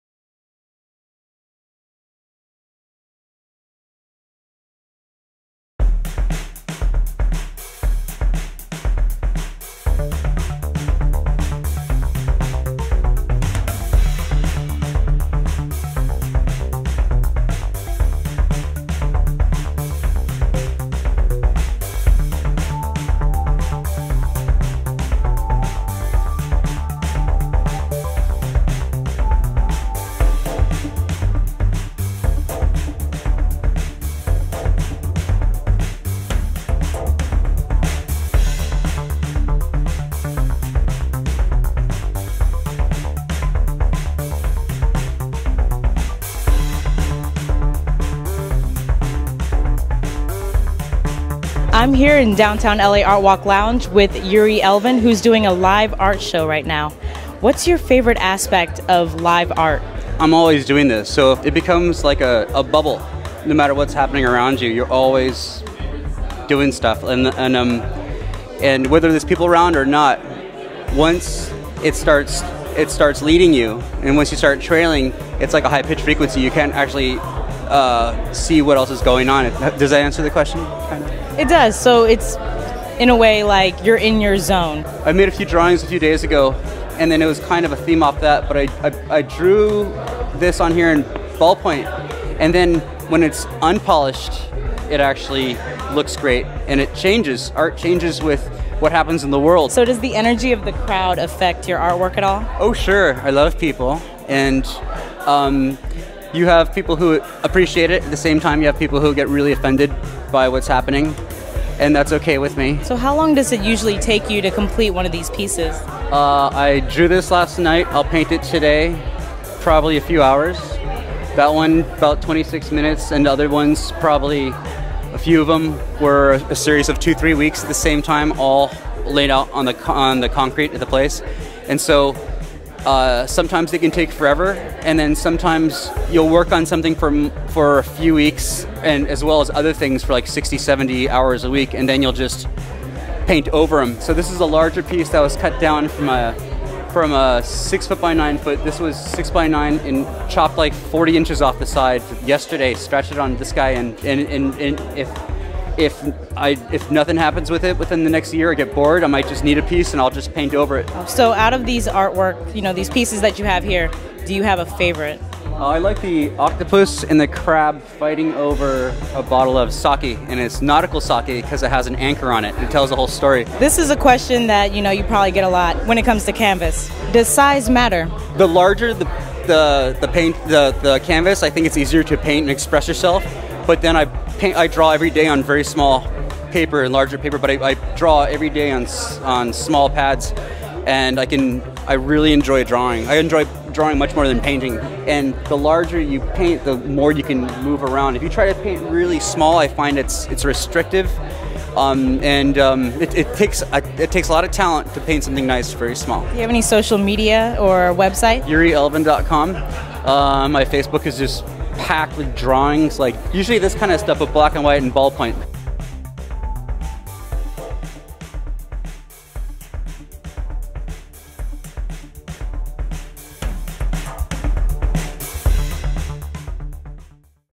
Downtown LA Art Walk January 2014
Come to DTLA Art Walk on the 2nd Thursday of EACH & EVERY month to enjoy some FREE Art, Music, Entertainment, walk the streets of DTLA to interact with the thousands of people who are coming out to enjoy a night on the town.

This is what happened at the Downtown Los Angeles Art Walk event in January 2014. Features live painter Yuri Elvin being interviewed by our DTLA Art Walk host Ashley Iola.

Also check out great art shows including MIA Gallery Blacklights.